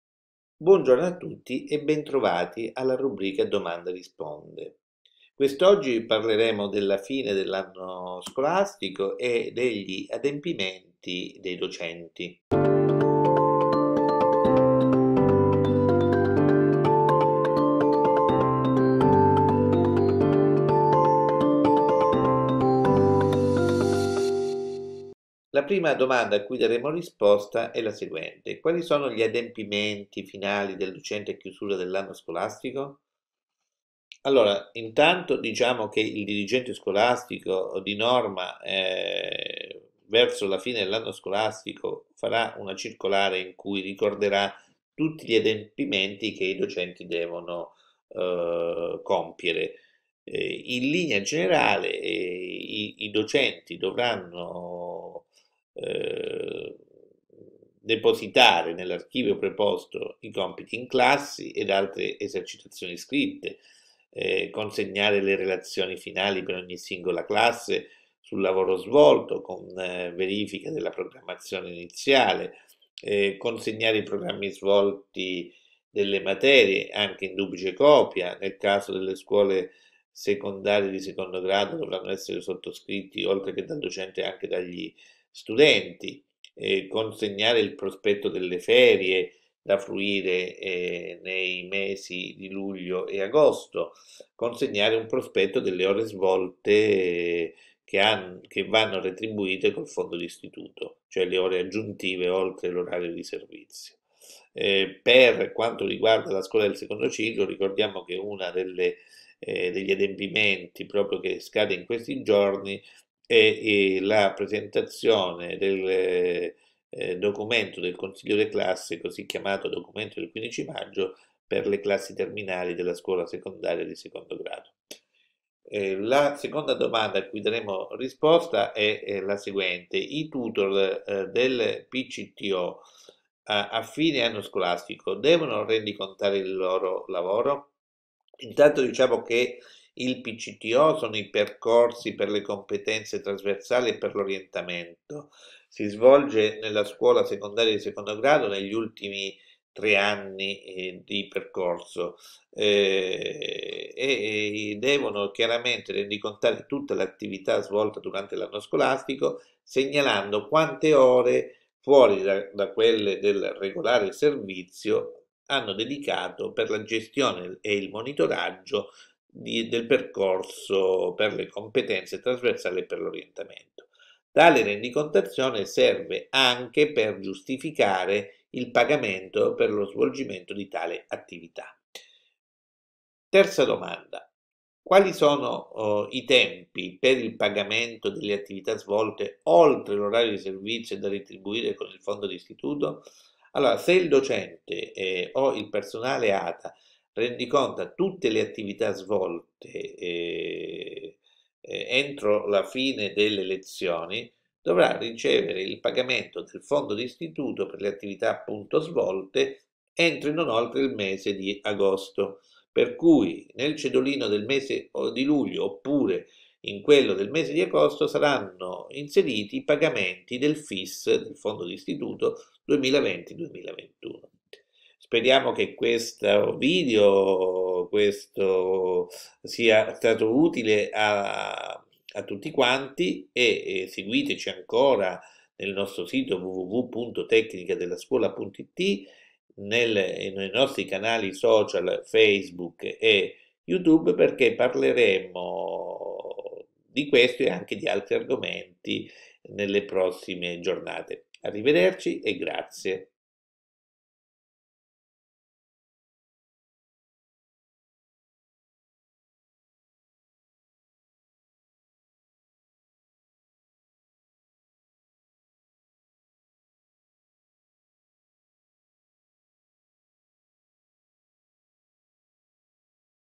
Fine scuola 2020/21: gli adempimenti dei docenti
finescuola notiziescuola tecnicadellascuola In questo appuntamento con la nostra rubrica “A domanda, risponde…” a cura del prof. Lucio Ficara, le risposte ai quesiti dei nostri lettori sul tema della fine dell'anno scolastico e degli adempimenti dei docenti.

Ecco i temi trattati:

1) Quali sono gli adempimenti finali del docente a chiusura dell’anno scolastico?

2) I tutor interni del PCTO, a fine anno scolastico, devono rendicontare il loro lavoro?

3) Quali sono i tempi per il pagamento delle attività svolte oltre l’orario di servizio e da retribuire con il fondo di Istituto?